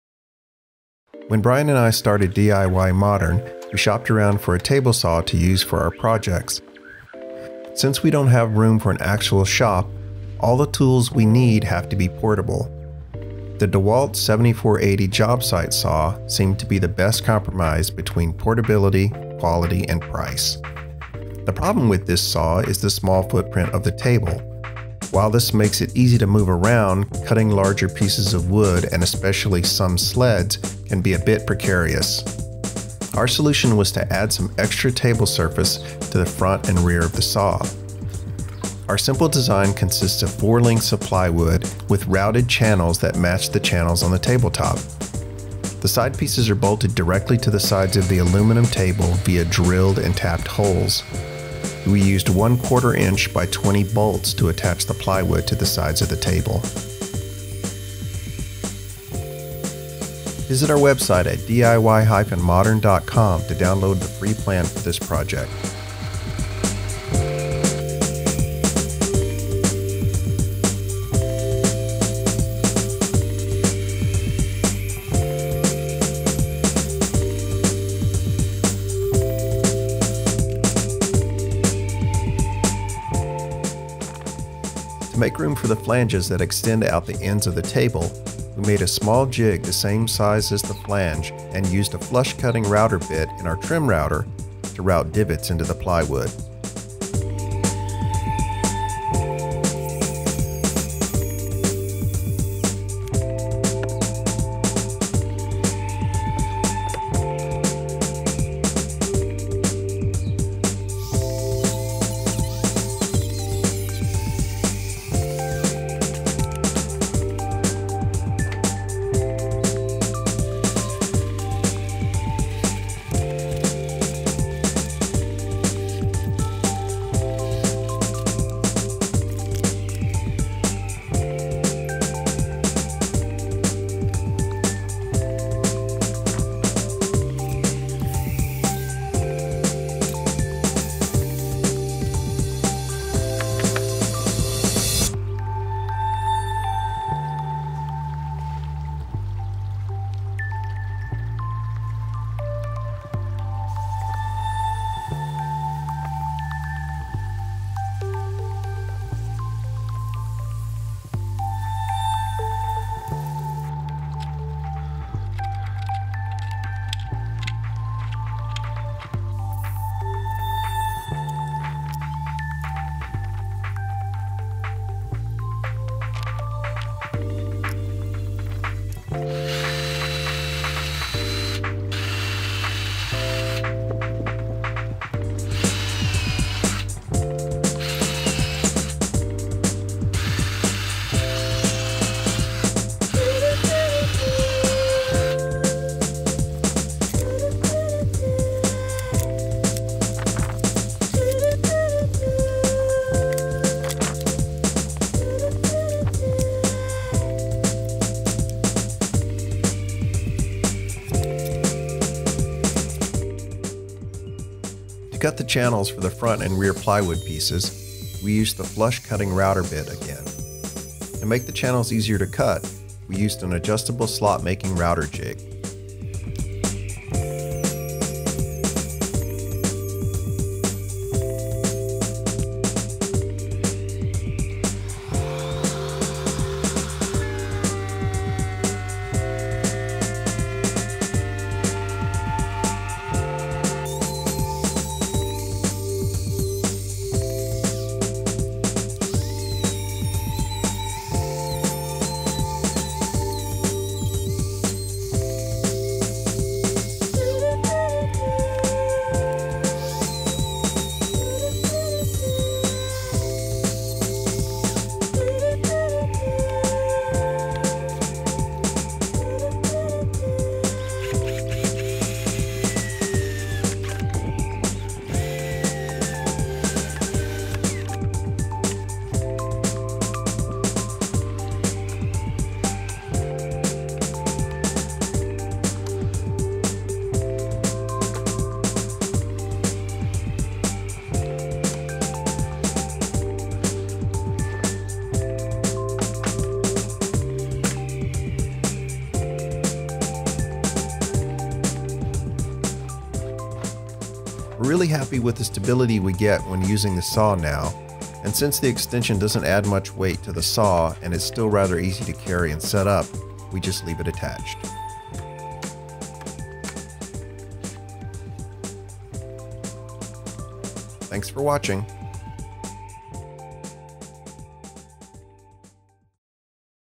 How to make a portable table saw extension for the DeWalt 7480
In this video we show how we added a table saw extension to our DeWalt 7480 table saw to improve stability while cutting longer pieces. to download the free plan for this project.